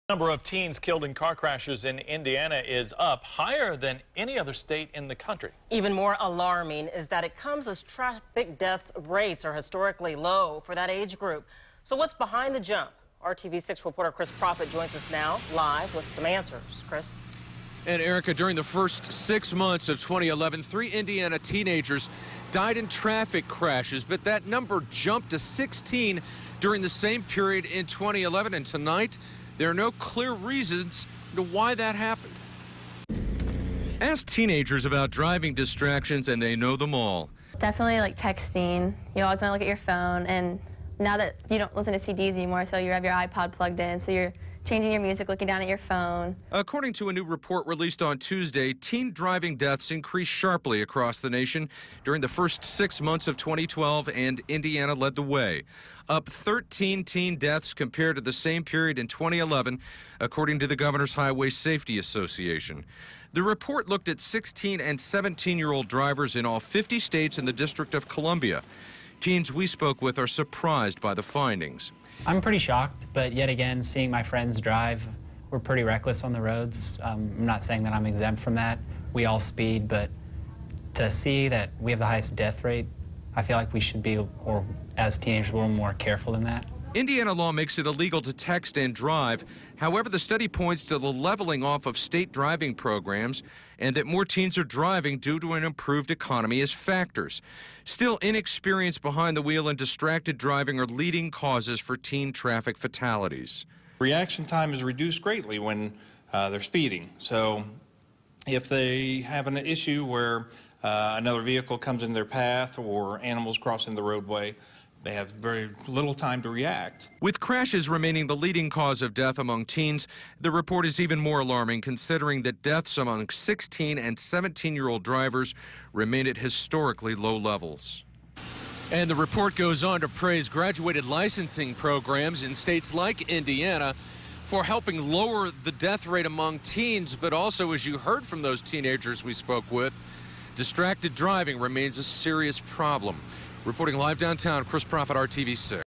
Report: Deaths among Indiana teen drivers highest in U.S.
The Governors Highway Safety Association reported that Indiana has the highest increase in the number of deaths of drivers between 16 and 17 years old in the United States.